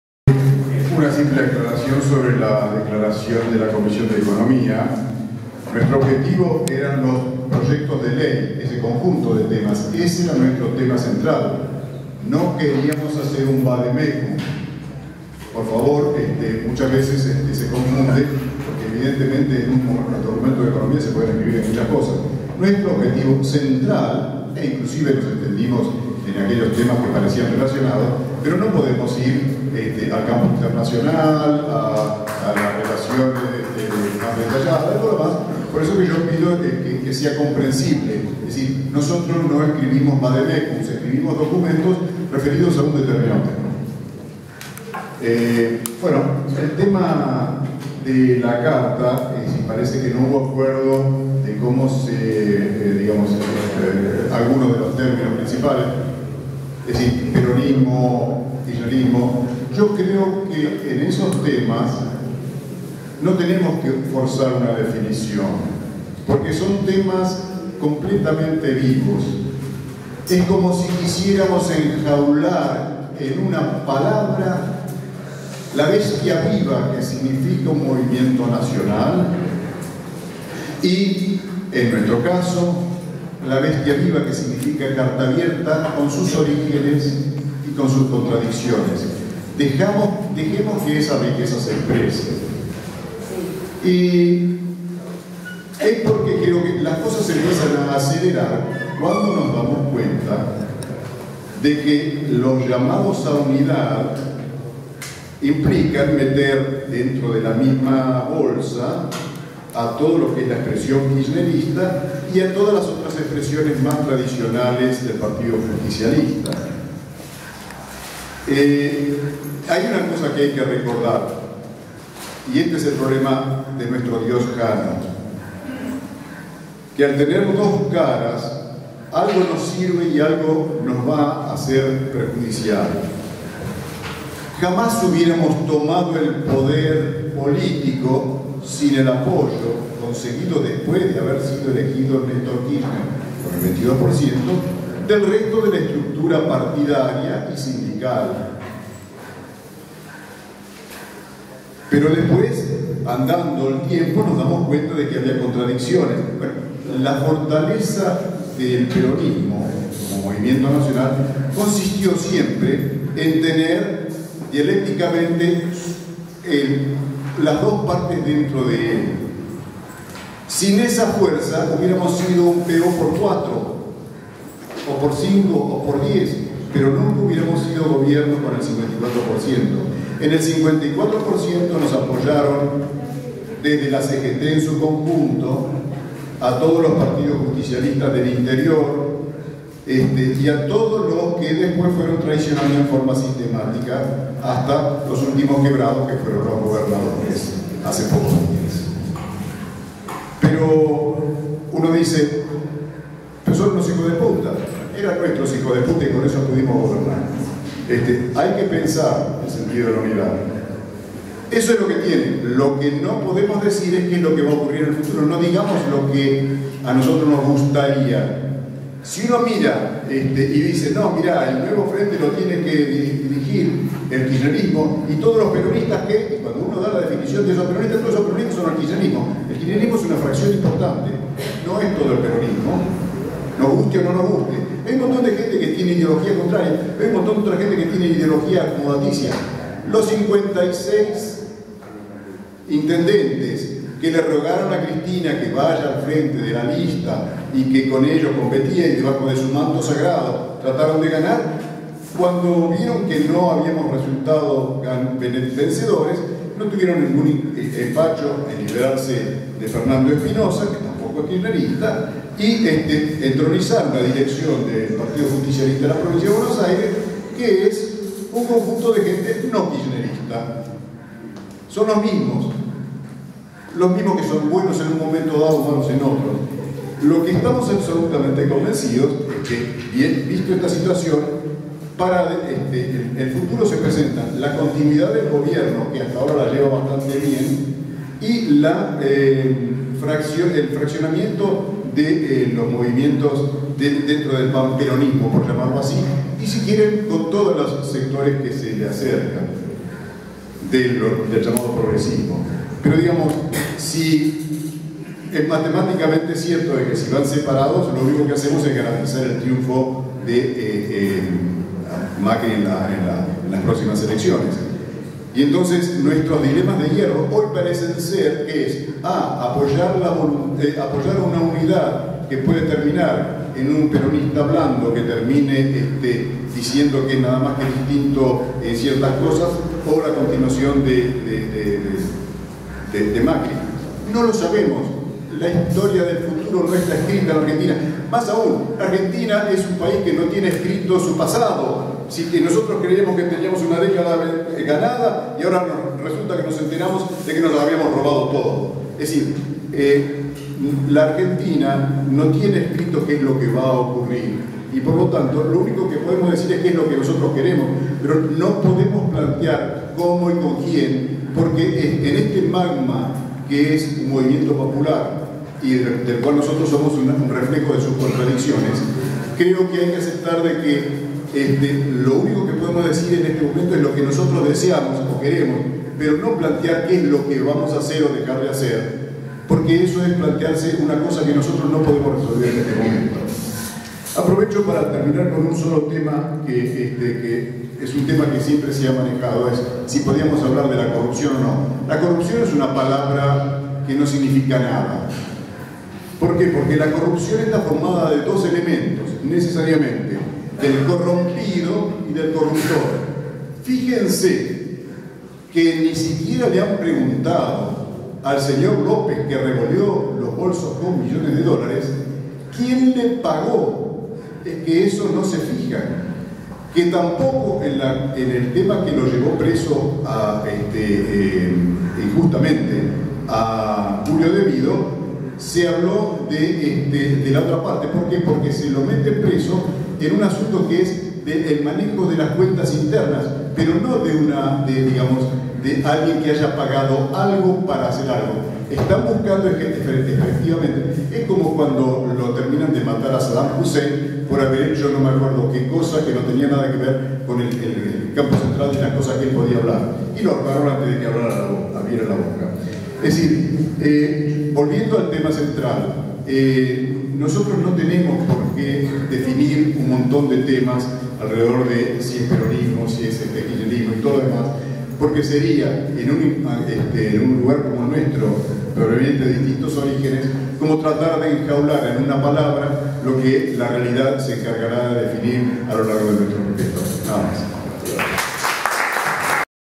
16CA Jorge Molinero
Asamblea de Carta Abierta del sábado 2 de diciembre de 2017.
Facultad de Ciencias Sociales. UBA
Discusión sobre puntos de desacuerdo para la redacción de la próxima Carta/25